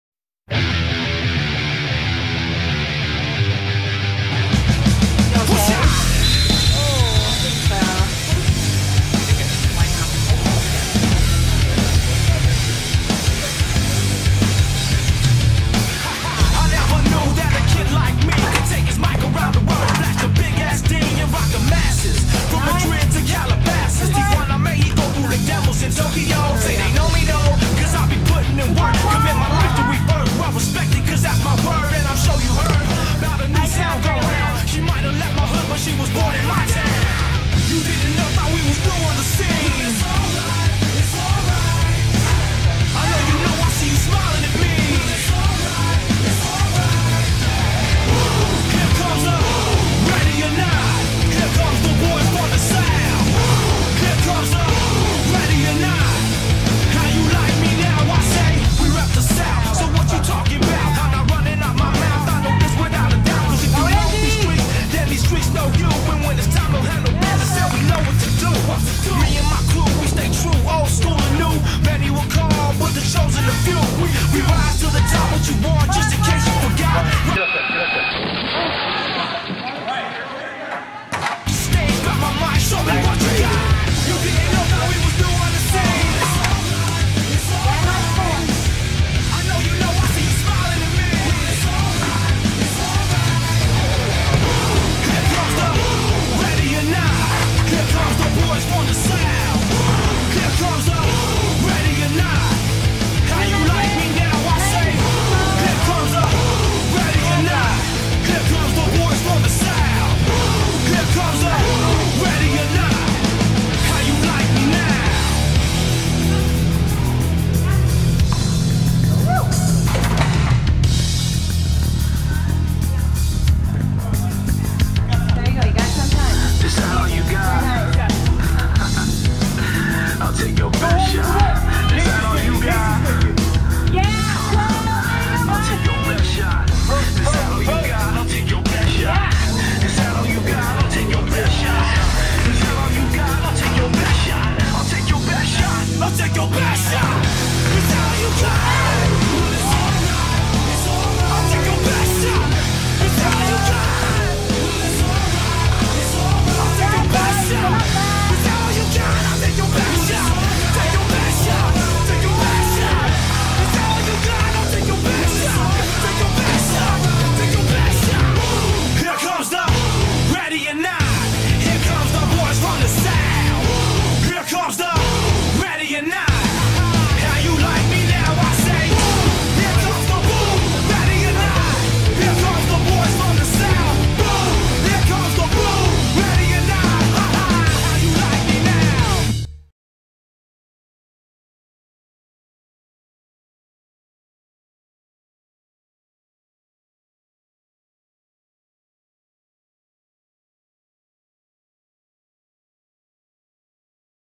Tualatin Indoor Soccer 2
highlights of our team in one of our games